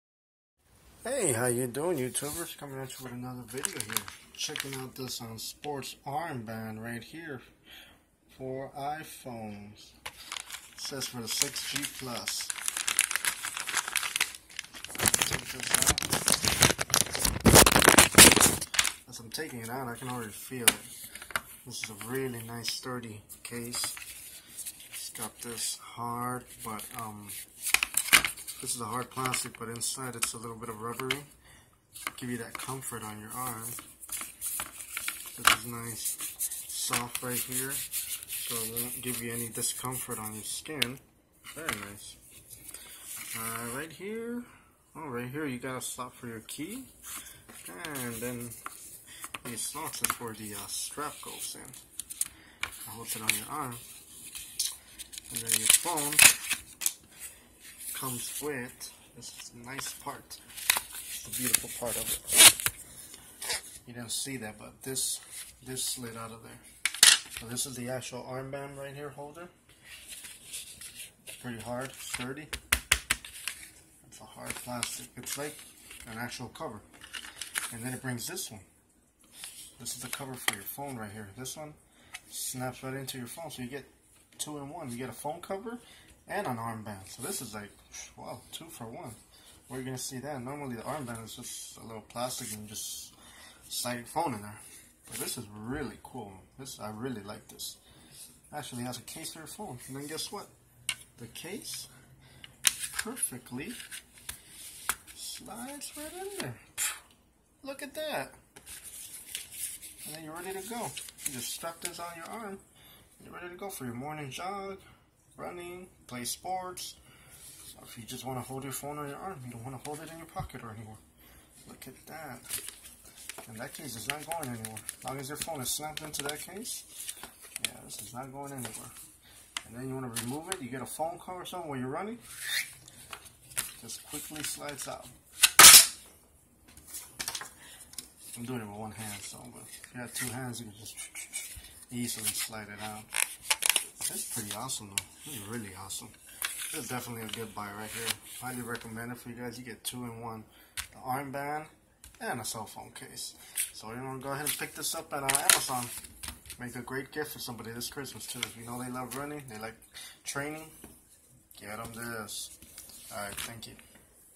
iPhone 6 plus armband case#RankBoosterReview ii. Sponsored iii. #(egrace)
Sports armband for iPhone 6 plus case great for jogging training or playing sports